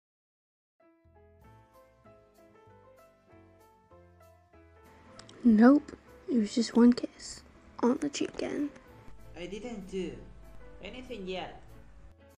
uzi kisses n on the cheek. MURDER DRONES (comic dub)
new nuzi video in this murder drones dub video.